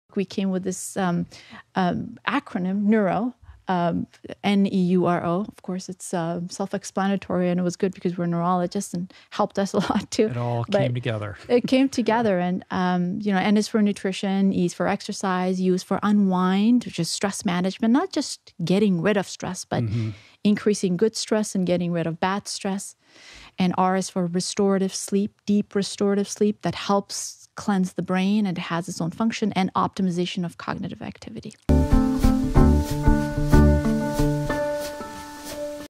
Unlock Your Brain's Potential with Neuro Academy: N.E.U.R.O
Discover how to unlock your brain's full potential with Neuro Academy - the ultimate platform for achieving optimal brain health and performance. Our acronym N.E.U.R.O. stands for Nutrition, Exercise, Unwind, Restorative Sleep, and Optimization of Cognition - the five pillars of brain health.